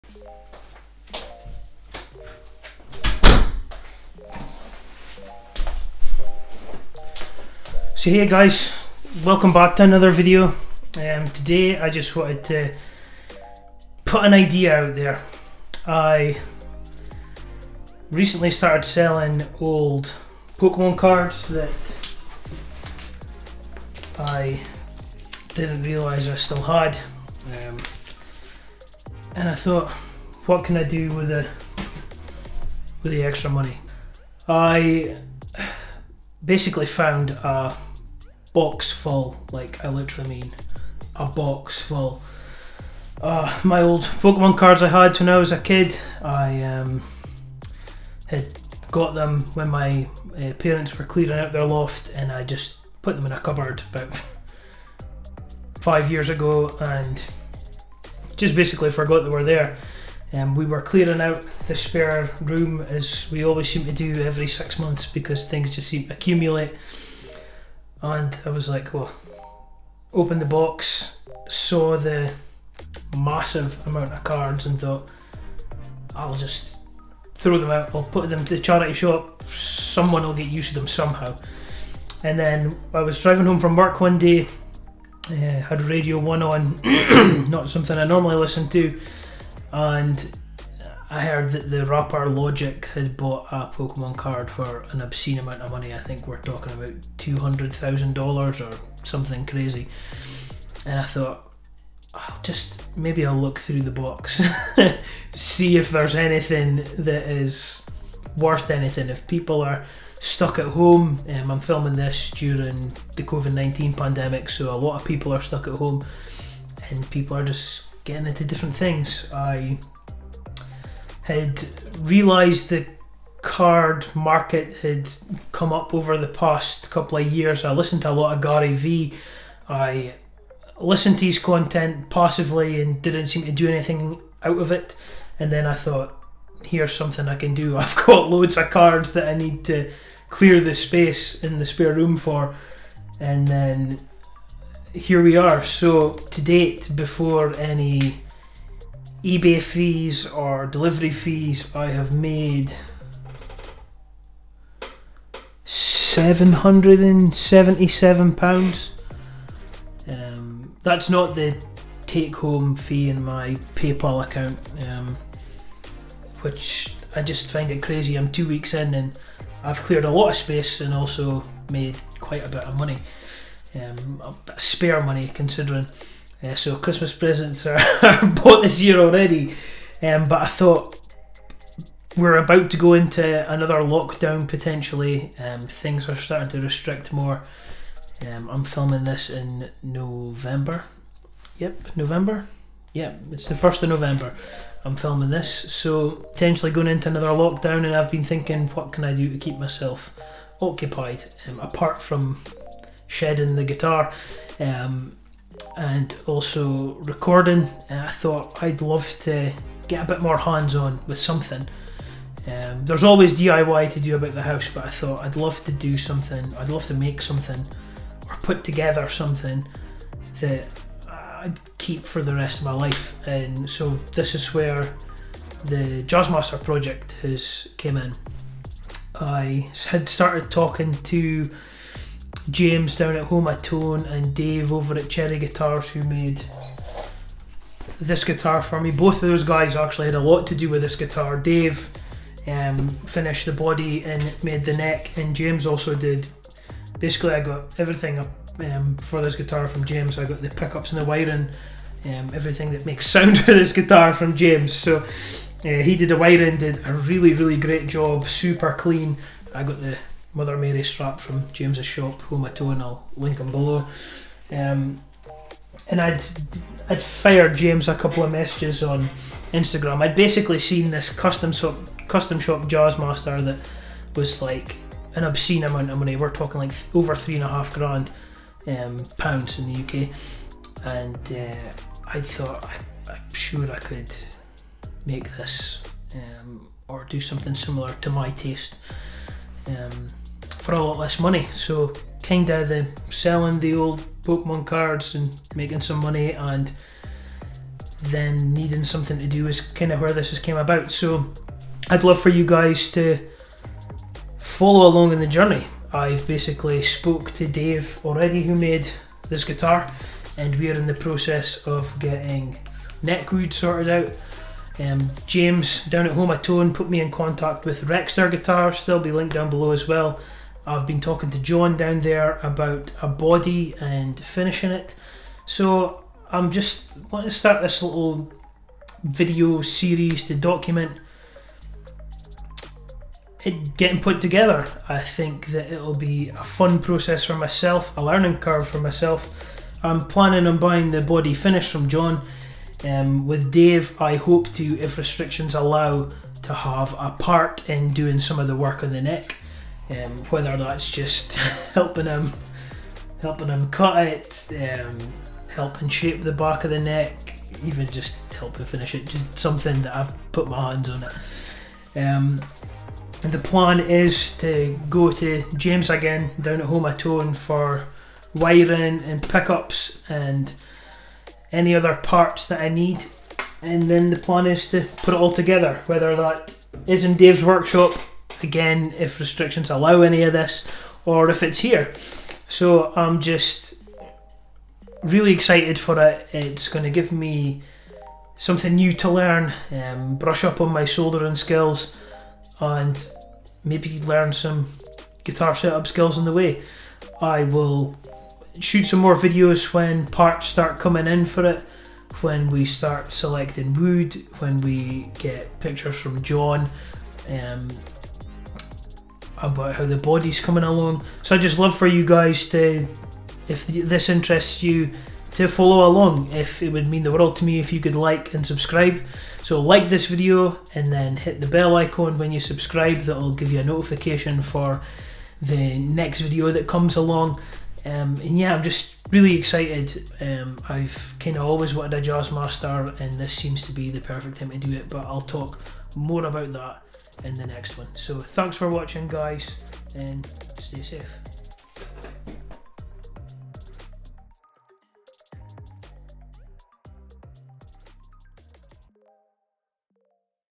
Turning old pokemon cards into my dream guitar - Jazzmaster Build Part 1
I recently found my old pokemon cards I've had for years ! decided to put them up on eBay following some advice from Gary Vee and I have turned them into cash ! which I plan on using to help make my dream guitar come to reality !

music from Amen Worldwide